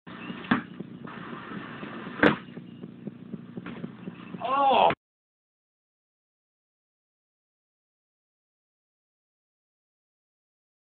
Basketball bounce shot fail. (2007)
Attempt at a nothing-but-net bounce shot. What could go wrong? **Lost video from July 2007.**